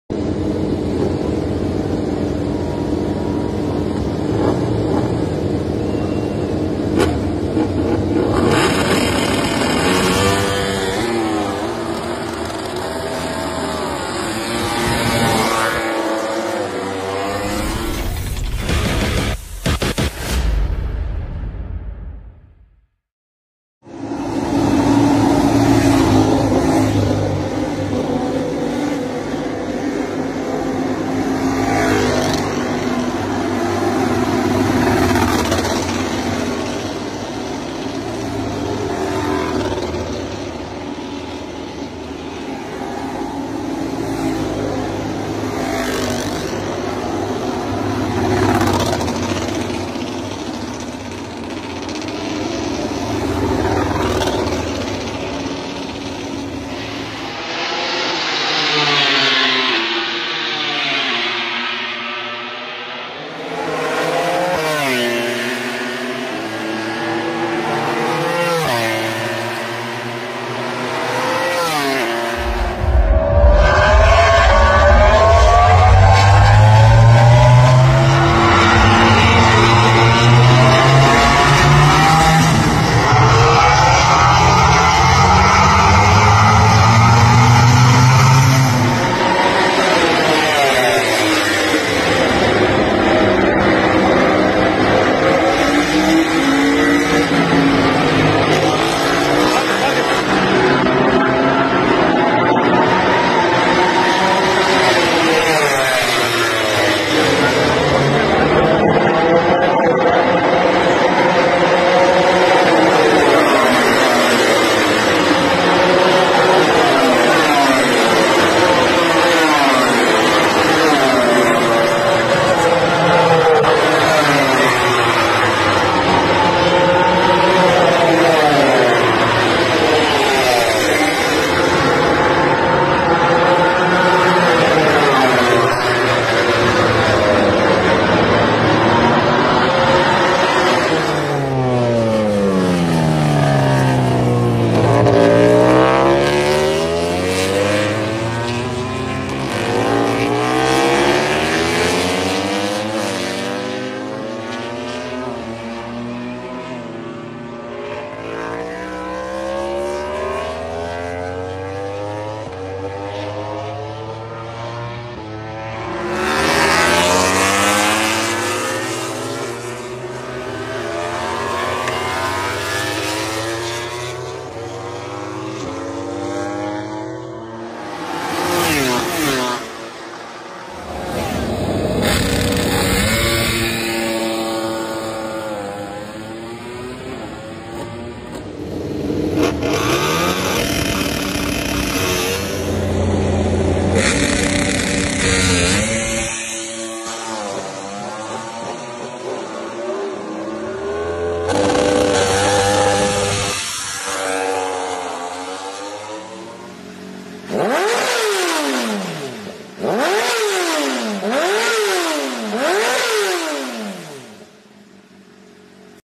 Flyby, practice start and pure sound Indian gp buddh international circuit
gelaran GP India 2023 sesi latihan bebas
sirkuit buddh